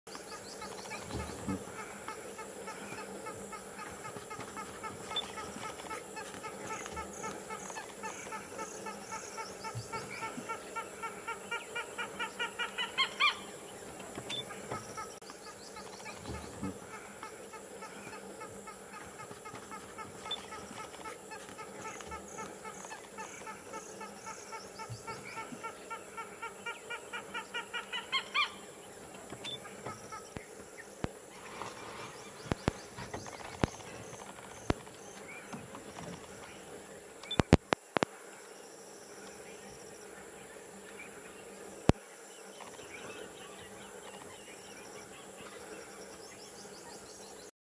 Peacock dancing to an audience of Malabar Pied Hornbills at dusk ,Willpattu National Park
Peacock dancing to an audience of Malabar Pied Hornbills ,Willpattu National Park